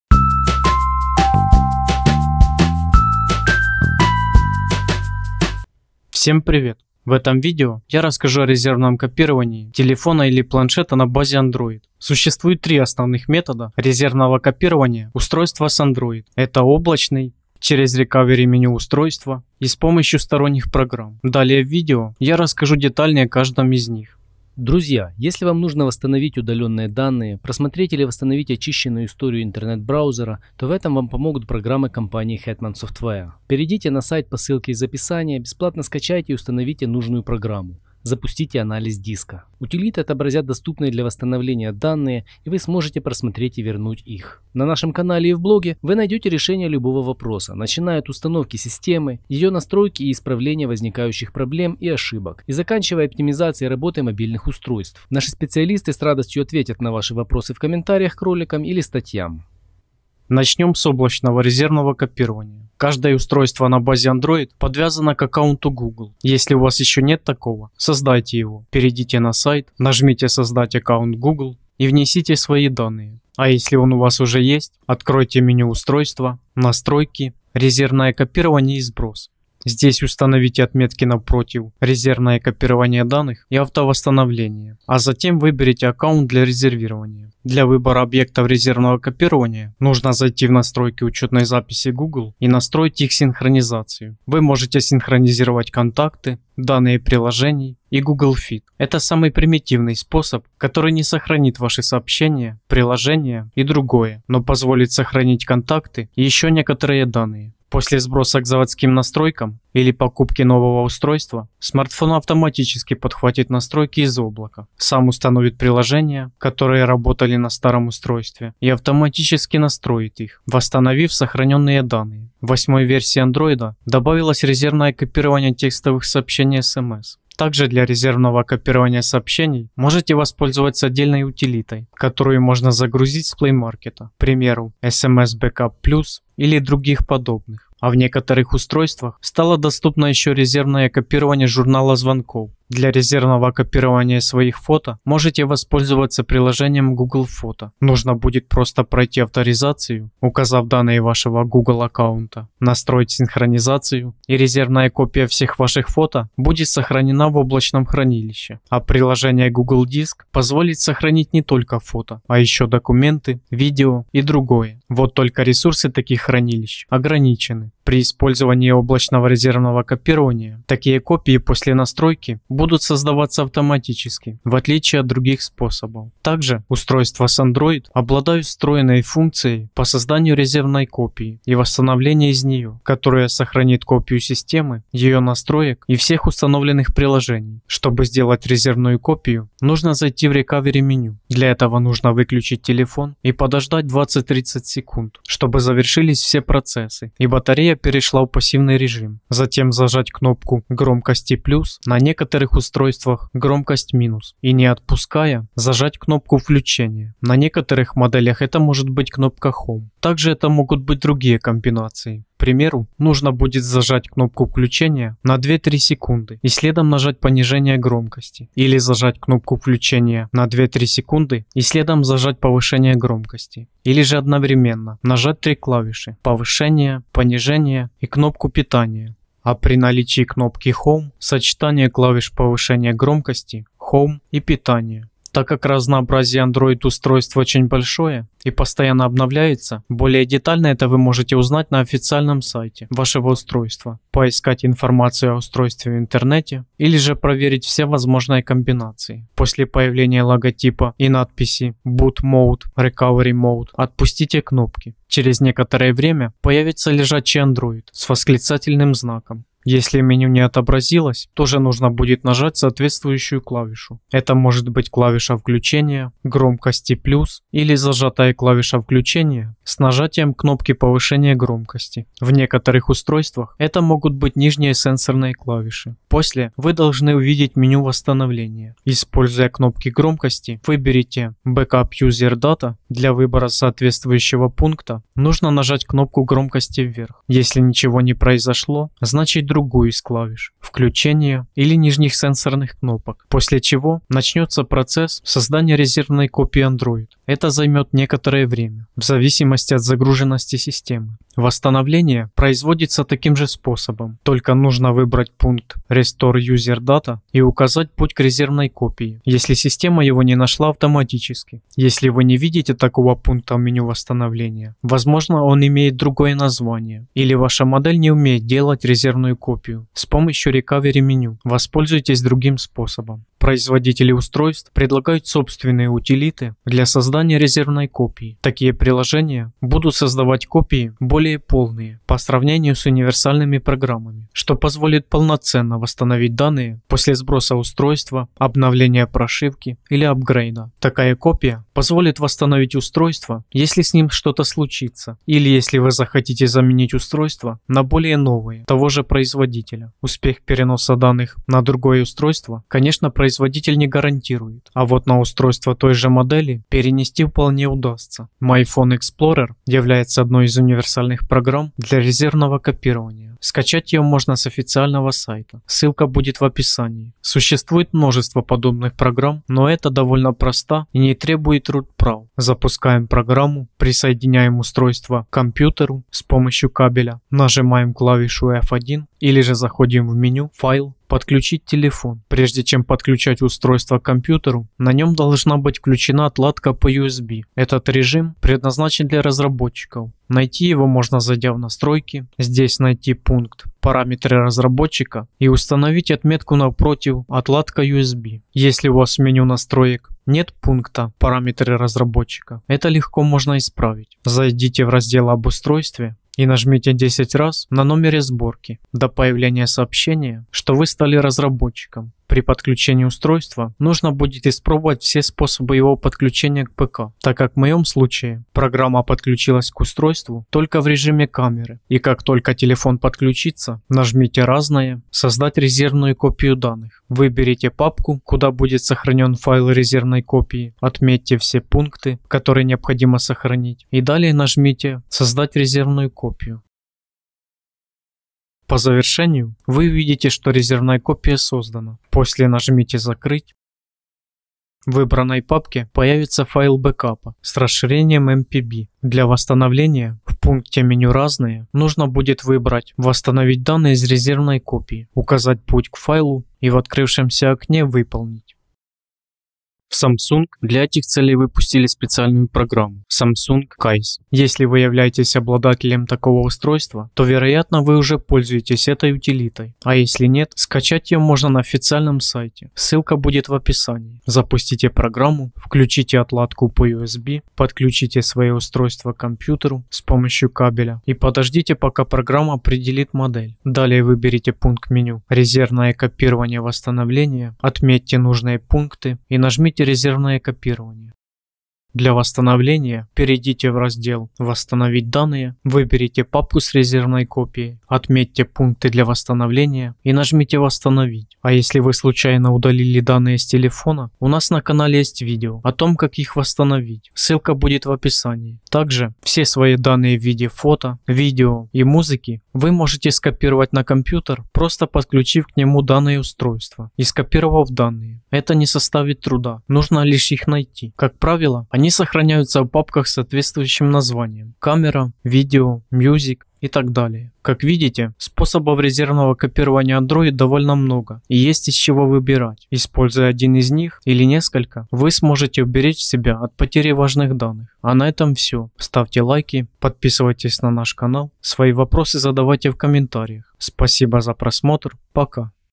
Резервное копирование Android (Облако ☁️, аккаунт Google 📱, программы для ПК 💻) в 2021
👍 Смотрите о резервном копировании телефона или планшета на базе Android. Существует 3 основных метода резервного копирования устройства с Android: облачный, через рекавери меню устройства, и с помощью сторонних программ (программный). Далее в видео я расскажу детальнее о каждом из них.

Содержание:
00:00 - Вступление;
1:01 - Облачное резервное копирование;
2:55 - Рекавери меню устройства;
5:21 - Программный способ;
7:39 - Для смартфонов и планшетов от Самсунга;

Как видите способов резервного копирования Android довольно много и есть из чего выбрать, используя один из них или несколько вы сможете уберечь себя от потери важных данных.

А на этом всё. Свои вопросы задавайте в комментариях. Пока.

Другие видео: РезервноеКопирование, Android, ТелефонАндроид, ПланшетAndroid.